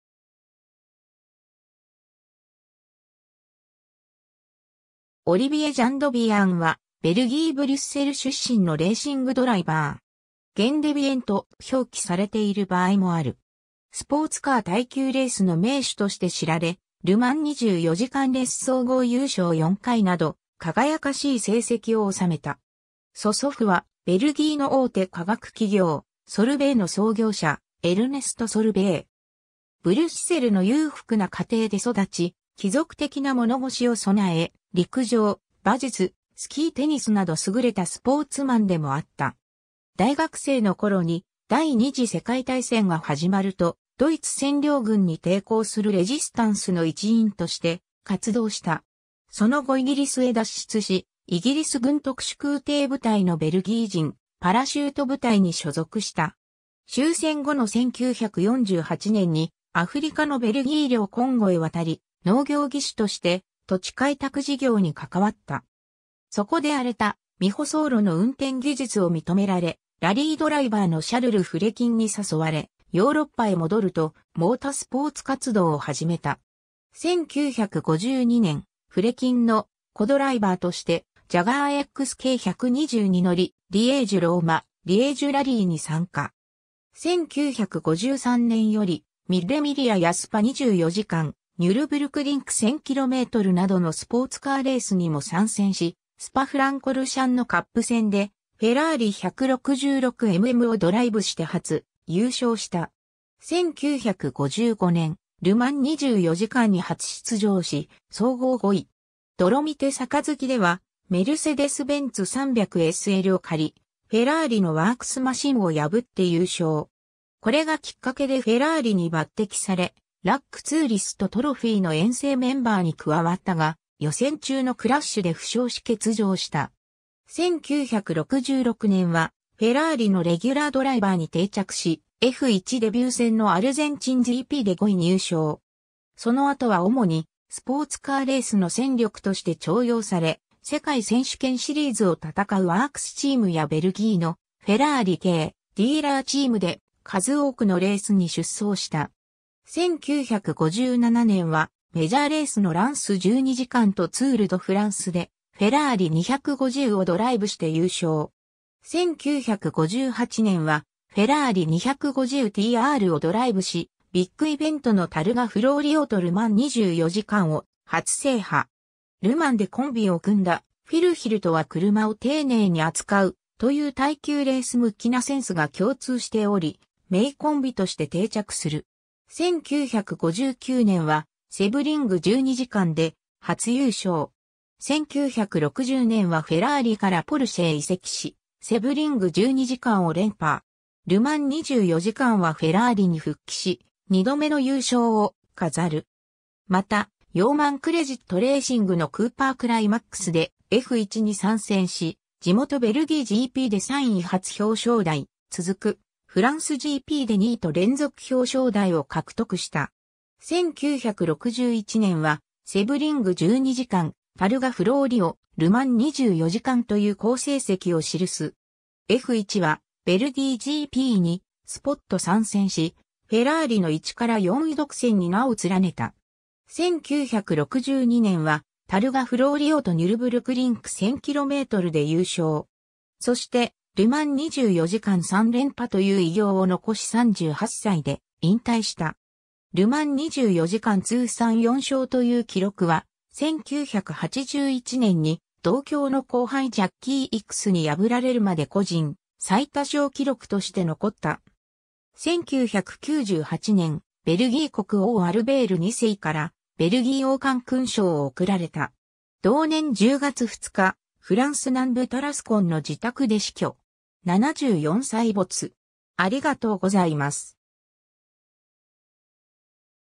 オリビエ・ジャンドビアン
ル・マン24時間勝者
タルガ・フローリオ勝者
オリヴィエ・ジャンドビアン（Olivier Gendebien 、1924年1月12日 - 1998年10月2日）は、ベルギーブリュッセル出身のレーシングドライバー。
「ゲンデビエン」と表記されている場合もある。
 スポーツカー耐久レースの名手として知られ、ル・マン24時間レース総合優勝4回など輝かしい成績を収めた。
 曽祖父（母方の祖母の父親）はベルギーの大手化学企業ソルベイの創業者エルネスト・ソルベイ。
ブリュッセルの裕福な家庭で育ち、貴族的な物腰を備え、陸上・馬術・スキー・テニスなど優れたスポーツマンでもあった。
大学生の頃に第二次世界大戦が始まると、ドイツ占領軍に抵抗するレジスタンスの一員として活動した。
その後イギリスへ脱出し、イギリス軍特殊空挺部隊 (SAS) のベルギー人パラシュート部隊に所属した。
 終戦後の1948年にアフリカのベルギー領コンゴへ渡り、農業技師として土地開拓事業に関わった。
そこで荒れた未舗装路の運転技術を認められ、ラリードライバーのシャルル・フレキン (Charles Fraikin) に誘われ、ヨーロッパへ戻るとモータスポーツ活動を始めた。
 1952年、フレキンのコ・ドライバーとしてジャガー・XK120に乗り、リエージュ-ローマ-リエージュ・ラリーに参加。
1953年よりミッレミリアやスパ24時間、ニュルブルクリンク1000kmなどのスポーツカーレースにも参戦し、スパ・フランコルシャンのカップ戦でフェラーリ・166MMをドライブして初優勝した。
 1955年、ル・マン24時間に初出場し総合5位。
ドロミテ杯ではメルセデス・ベンツ 300SLを駆り、フェラーリのワークスマシンを破って優勝。
これがきっかけでフェラーリに抜擢され、RAC ツーリスト・トロフィーの遠征メンバーに加わったが、予選中のクラッシュで負傷し欠場した。
 1966年はフェラーリのレギュラードライバーに定着し、F1デビュー戦のアルゼンチンGPで5位入賞。
その後はおもにスポーツカーレースの戦力として重用され、世界選手権シリーズを戦うワークスチーム（スクーデリア・フェラーリ）やベルギーのフェラーリ系ディーラーチーム（エキュリー・フランコルシャンやエキュリー・ナショナル・ベルジュ）で数多くのレースに出走した。
 1957年はメジャーレースのランス12時間 (12 Hours of Reims) とツール・ド・フランス (Tour de France) でフェラーリ・250をドライブして優勝。
1958年はフェラーリ・250TRをドライブし、ビッグイベントのタルガ・フローリオとルマン24時間を初制覇。
ル・マンでコンビを組んだフィル・ヒルとは”車を丁寧に扱う”という耐久レース向きなセンスが共通しており、名コンビとして定着する。
1959年はセブリング12時間で初優勝。
 1960年はフェラーリからポルシェへ移籍し、セブリング12時間を連覇。
ル・マン24時間はフェラーリに復帰し、2度目の優勝を飾る。
また、ヨーマン・クレジット・レーシング（BRP）のクーパー・クライマックスでF1に参戦し、地元ベルギーGPで3位初表彰台、続くフランスGPで2位と連続表彰台を獲得した（シリーズランキング6位）。
 1961年はセブリング12時間（3連覇）、タルガ・フローリオ（2勝目）、ル・マン24時間（3勝目）という好成績を記す。
F1はベルギーGPにスポット参戦し、フェラーリの1-4位独占（ヒル-フォン・トリップス-ギンサー-ジャンドビアン）に名を連ねた。
 1962年はタルガ・フローリオ（3勝目）とニュルブルクリンク1000kmで優勝。
そして、ル・マン24時間3連覇という偉業を残し38歳で引退した。
ル・マン24時間通算4勝（ヒルとのコンビでは3勝）という記録は、1981年に同郷の後輩ジャッキー・イクスに破られるまで個人最多勝記録として残った。
 1998年、ベルギー国王アルベール2世からベルギー王冠勲章 (Order of the Crown) を贈られた。
同年10月2日、フランス南部タラスコンの自宅で死去。
74歳没。